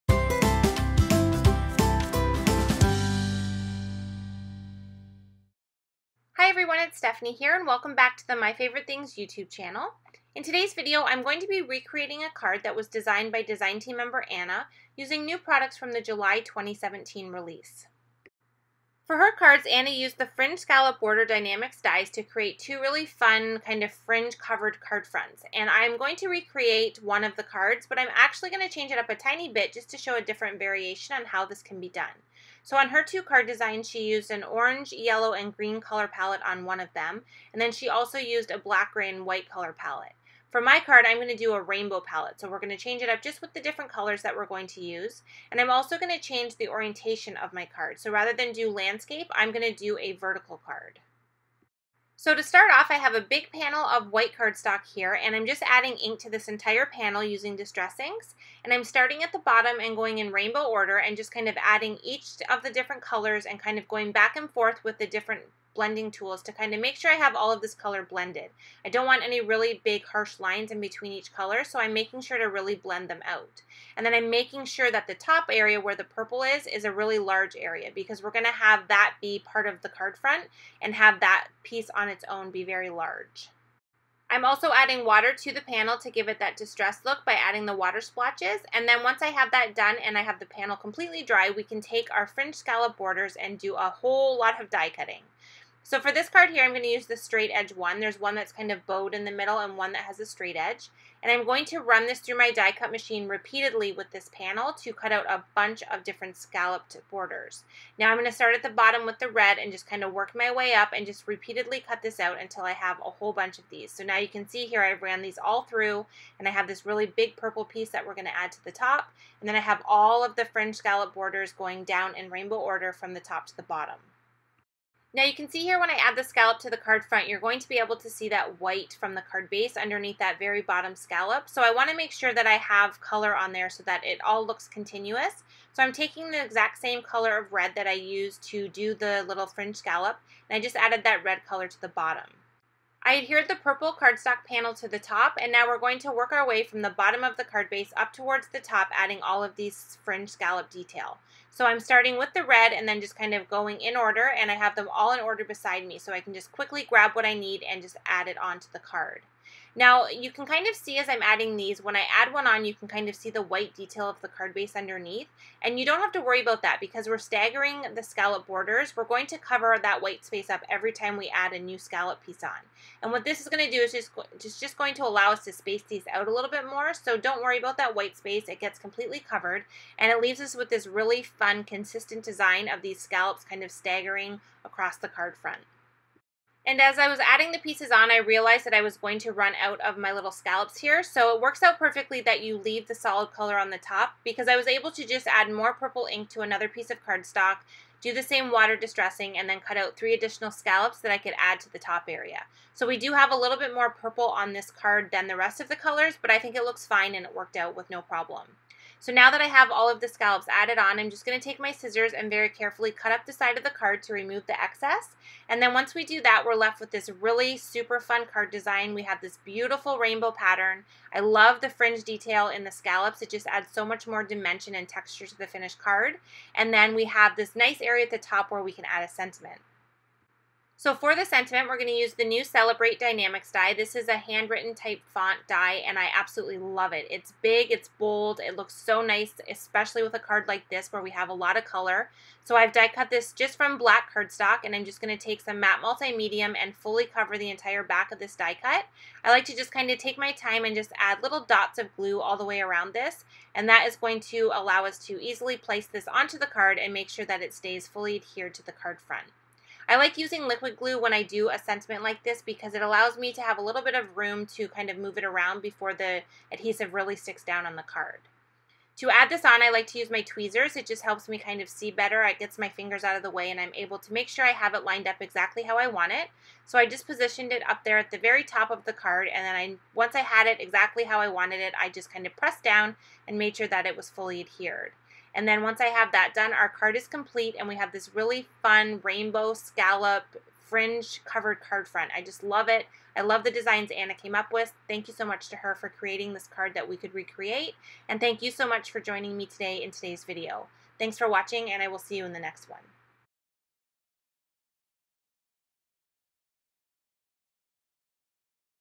July 2017 Release Class - Fringed Front
Follow along as Stephanie re-creates a project created by design team member Anna, using products from the new July 2017 release.